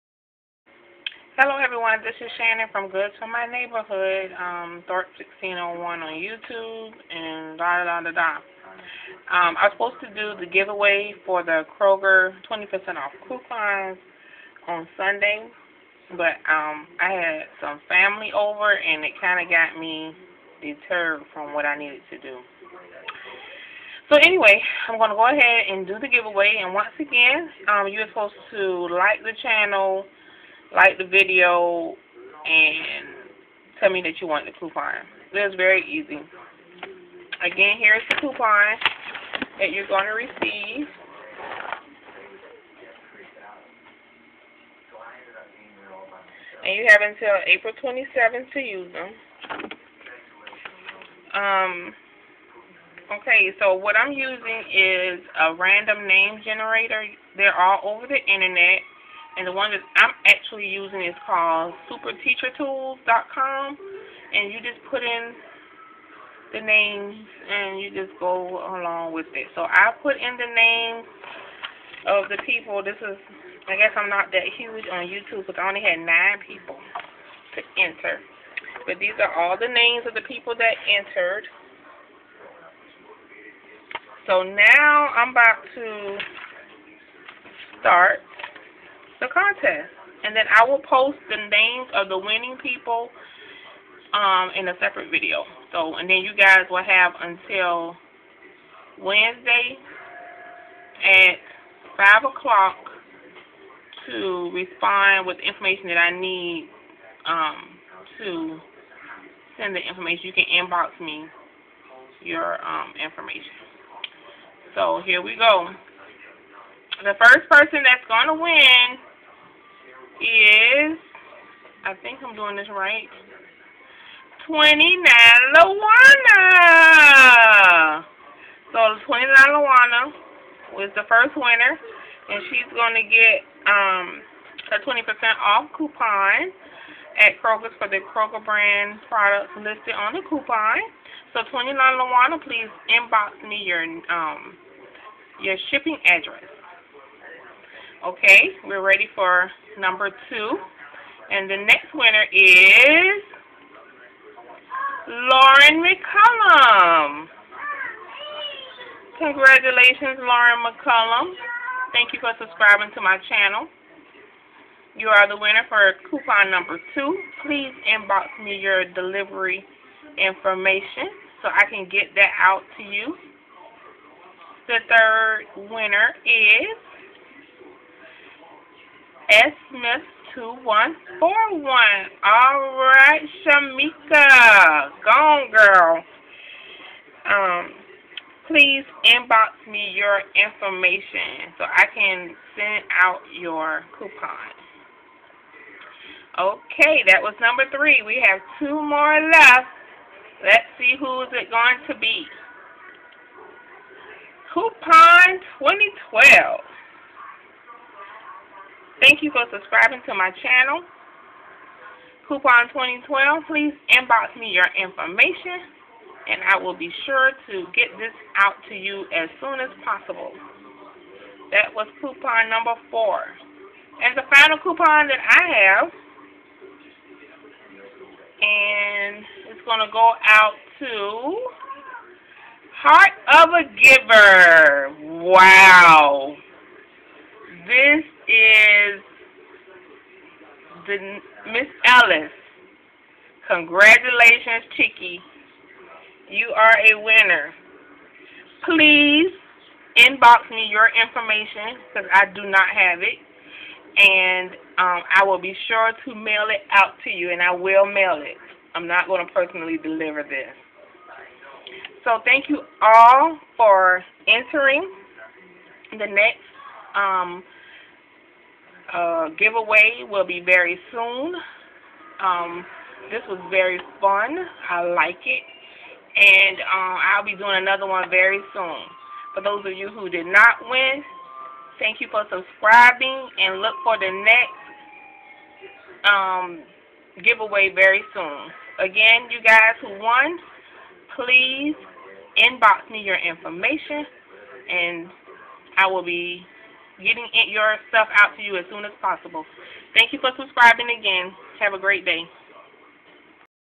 Kroger Bzzagent Giveaway Winners Announced
CONGRATULATIONS TO THE FOLLOWING WINNERS:
COUPON2012
LOREN MCCULLUM
HEARTOFAGIVER
29LAWANNA
SSMITH2141
***PLEASE INBOX ME YOUR MAILING INFORMATION BY WEDNESDAY 3/27/2013***